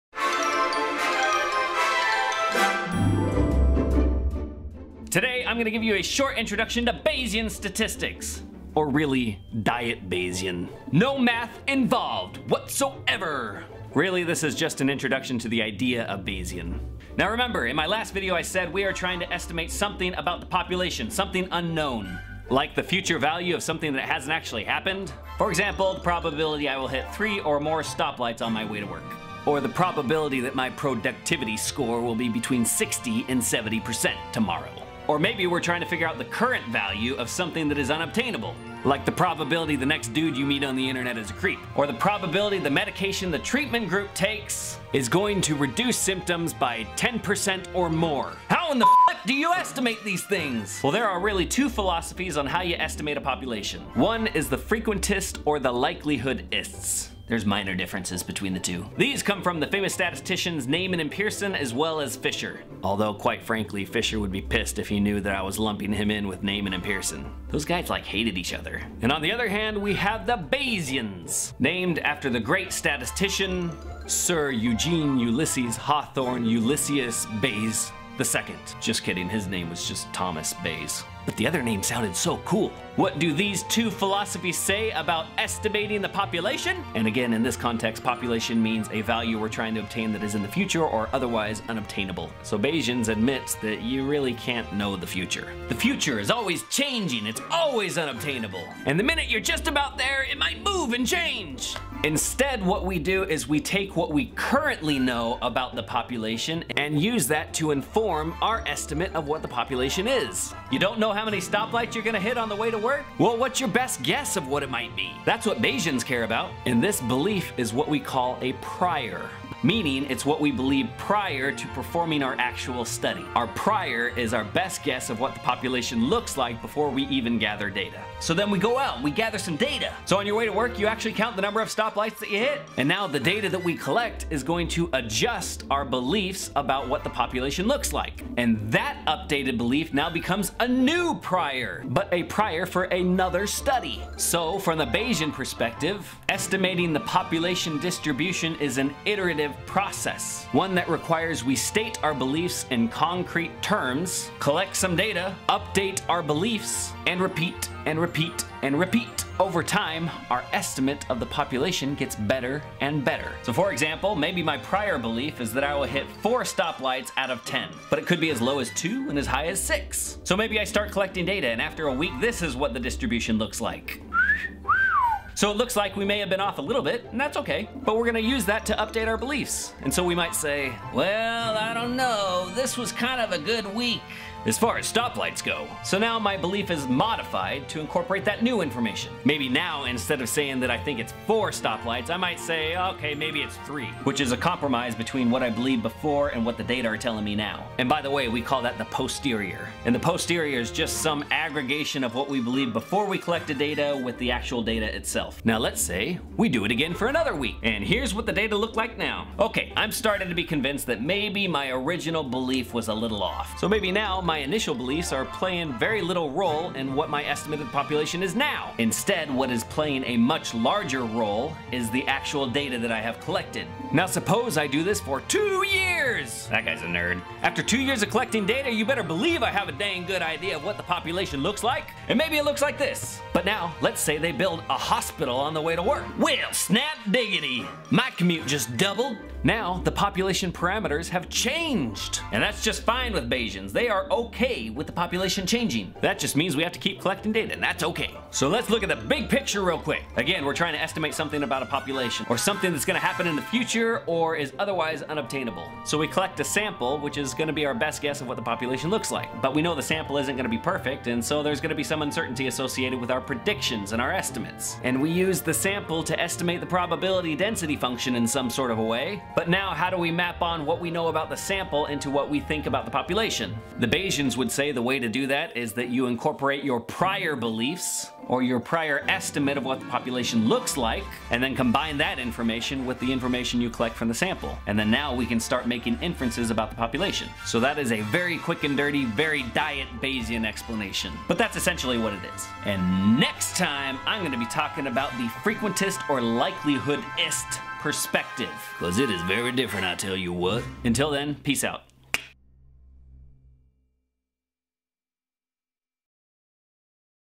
Probability, Part 4: Super Simple Explanation of Bayesian Statistics for Dummies
Learning objectives:

Understand a prior
Understand a posterior
Understand the role of subjective beliefs
Understand the bayesian approach to estimating the population